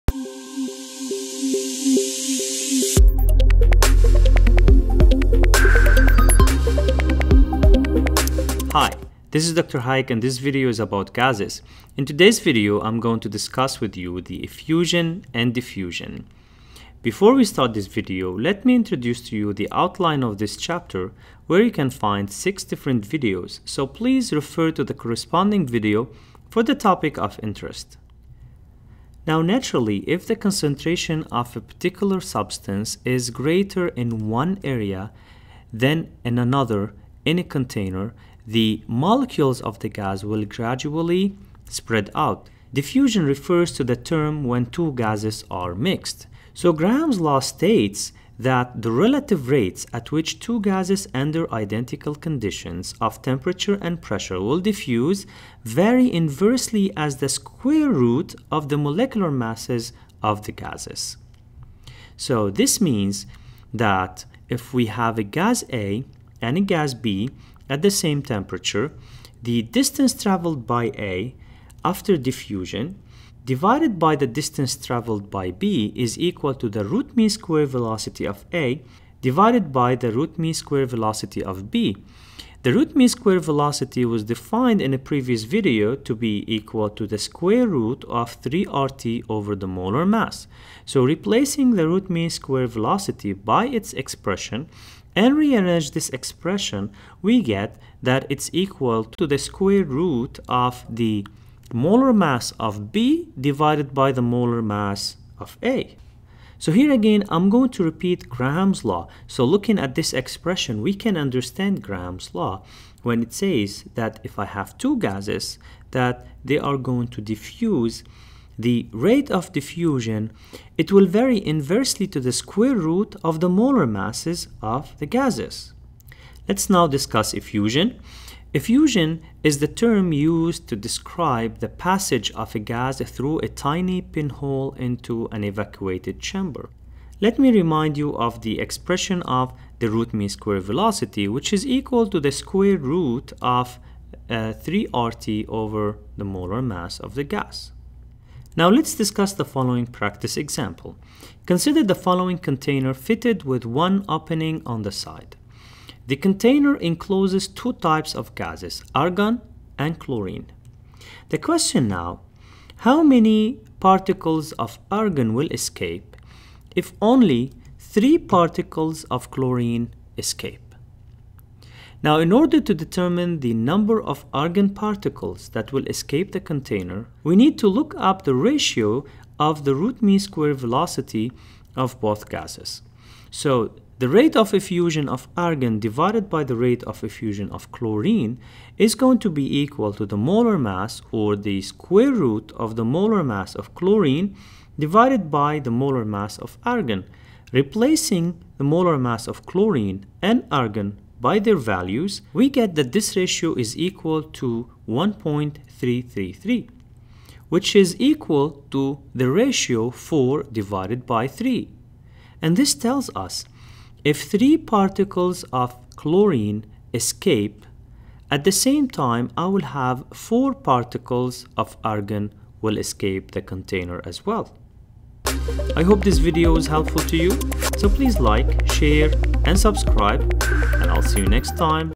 Gases | Effusion & Diffusion.
This video is about Gases and explains in details the effusion and diffusion processes.

Moreover, in this video, you will learn about:

- Graham's law.
- Rate of diffusion.
- Rate of effusion.

Students studying Chemistry at different levels could highly benefit from this video.

This video is the fourth of a complete series of six videos that explains different topics on Gases that include: